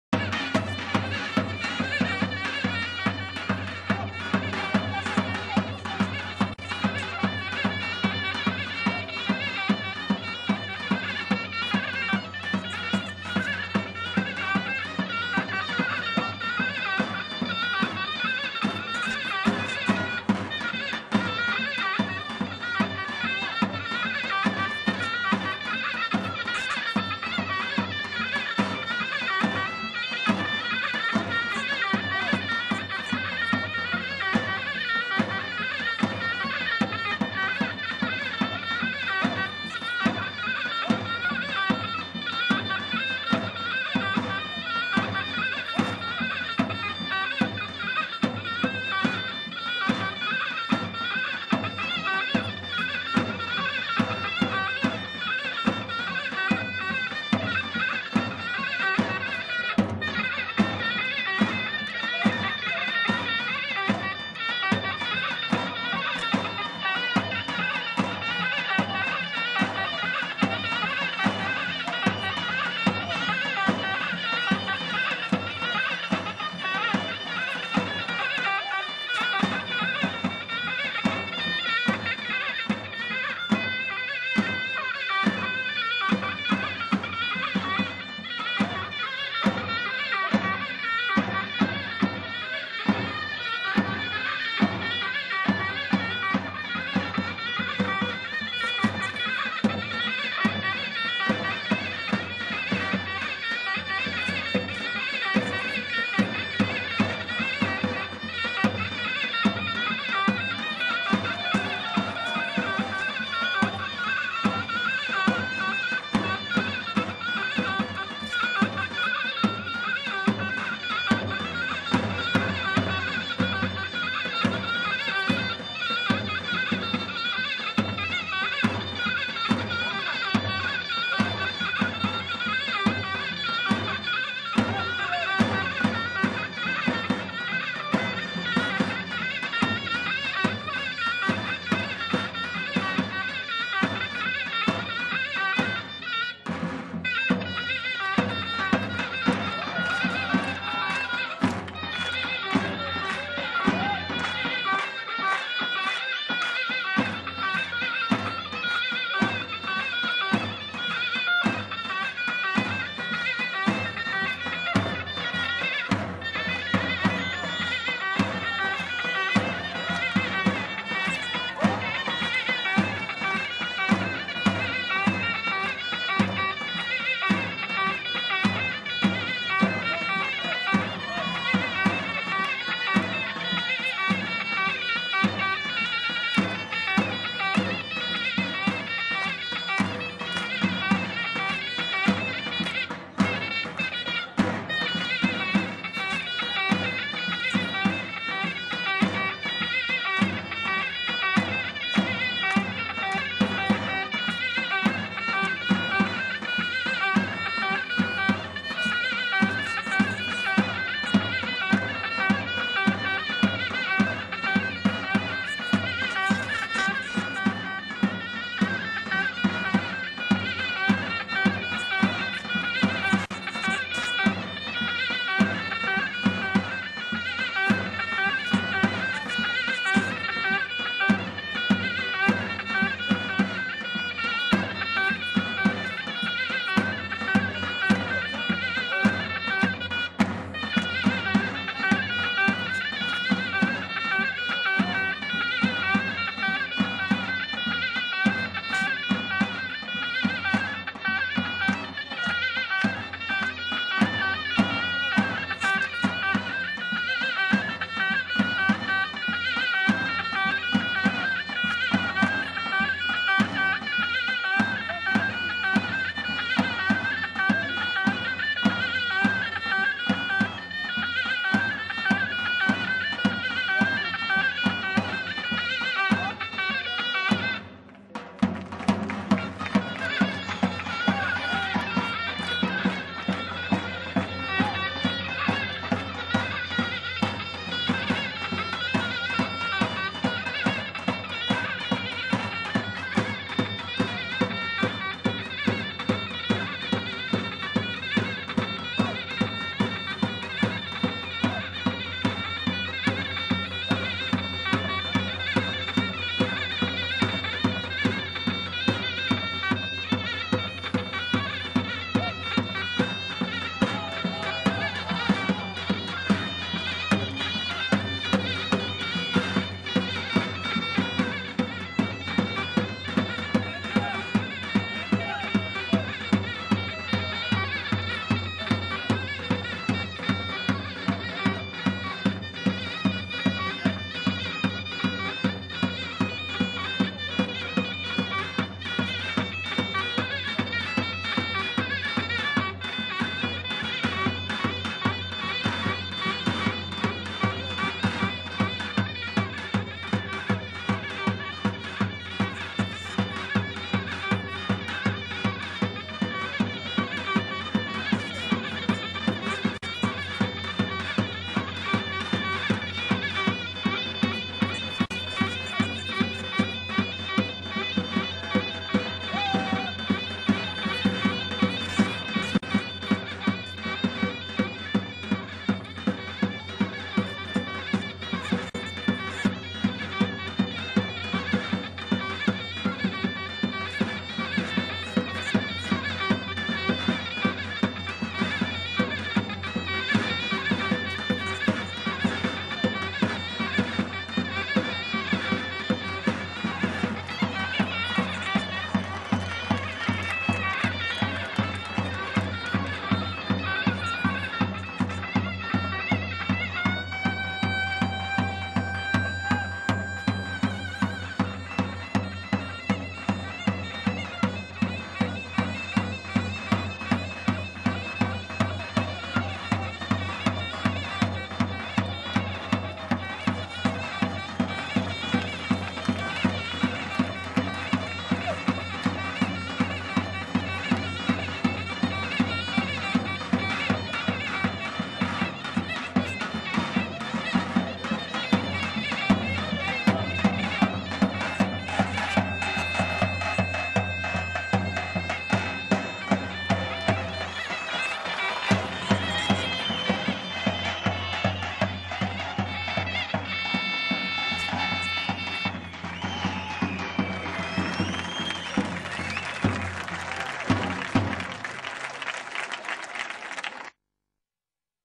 Ankara Haymana Balçıkhisar HALK OYUN EKİBİ-2
Evet arkadaslar bu ekibimizin devami 3 halay çesitimiz var yine nare zermercan ve hoplama çesitlerii  i izlemeler...